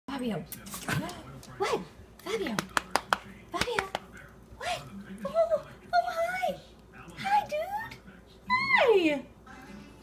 Meet Fabio a Papillon currently available for adoption at Petango.com! 10/29/2014 10:30:57 PM
This handsome gentleman is Fabio. He is an eight year old Papillon who is looking for a home after his previous owners moved and were unable to take him along. Fabio can be a bit shy initially, but once he finds someone he trusts, you get to see his sweet and gentle personality. 11 pound Fabio is quite handsome and sports a luxuriously soft multi-colored coat. He hasn't been around cats before, but has lived with other dogs successfully. Fabio is looking for a quieter home with teenagers and adults, that can give him extra time to adjust and settle in. If you have room in your home for sweet little Fabio, please stop in and meet him soon!